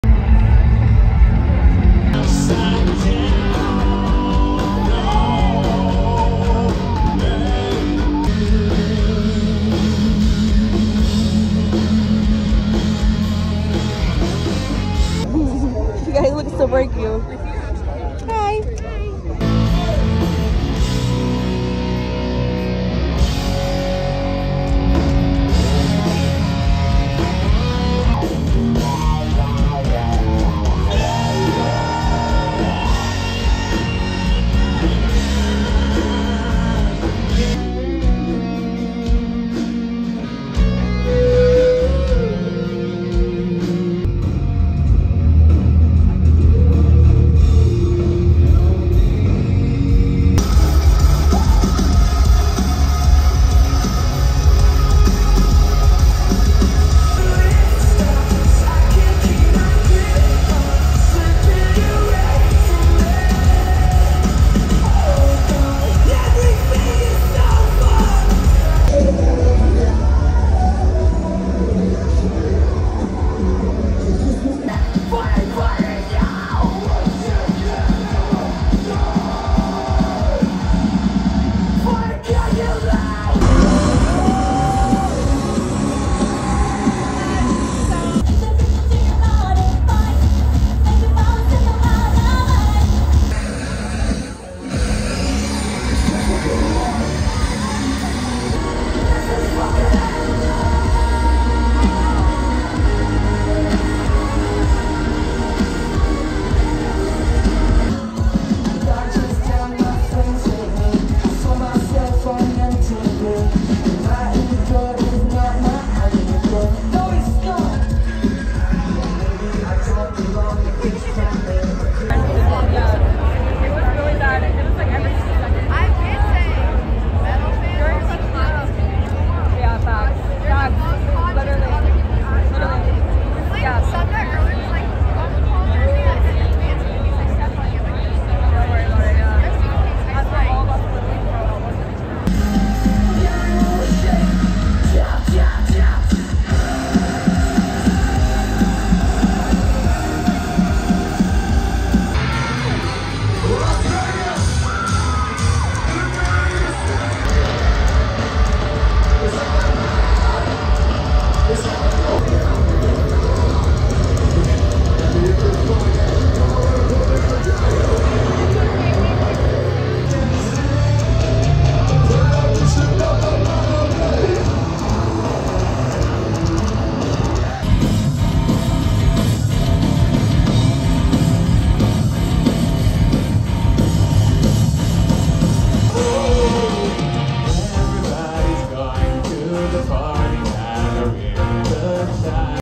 pov: sick new world • alice in chains, bring me the horizon & slipknot
pov: sick new world • alice in chains, bring me the horizon & slipknot are the full performances live i saw at the sick new world music festival that happened in the las vegas festival grounds, 2024